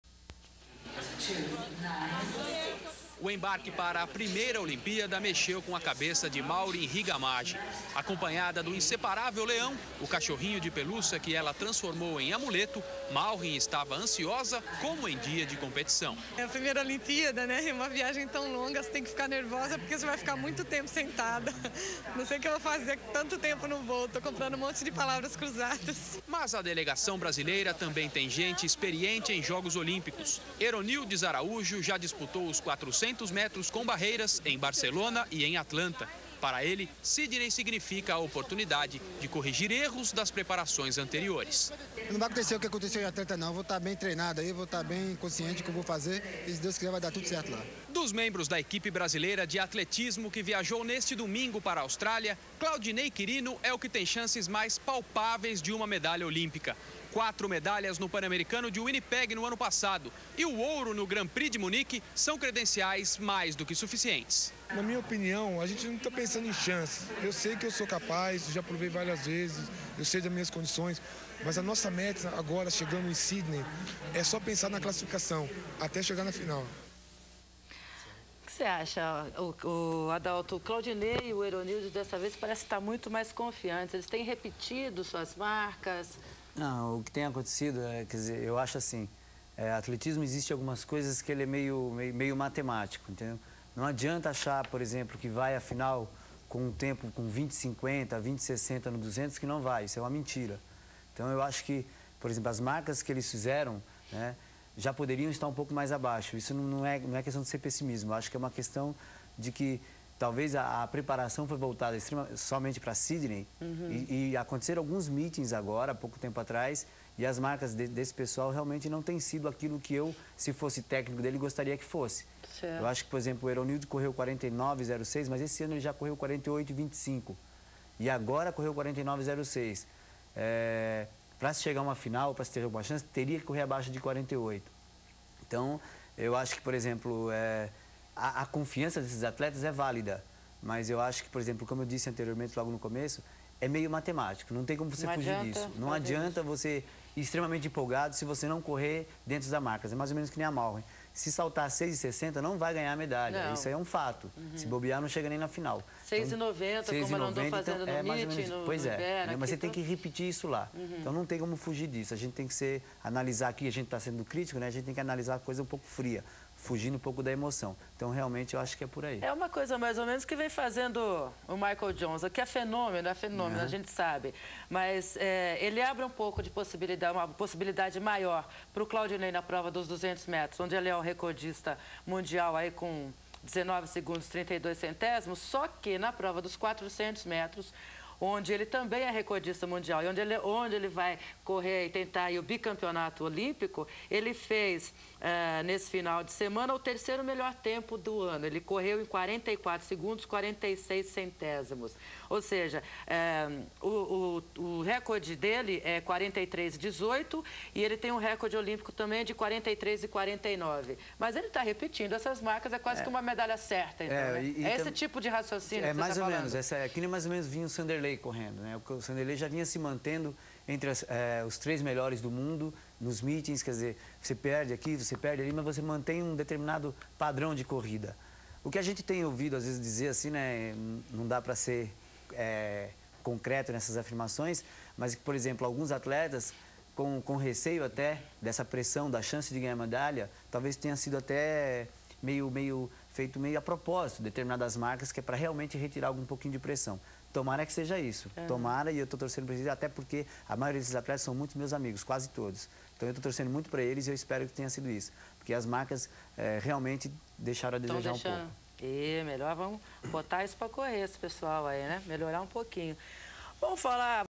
Embarque da equipe brasileira de Atletismo para Sidney - 2000
Claudinei Quirino, Maurren Maggi e Eronildes Nunes Araújo. Comentários de Adauto Domingues.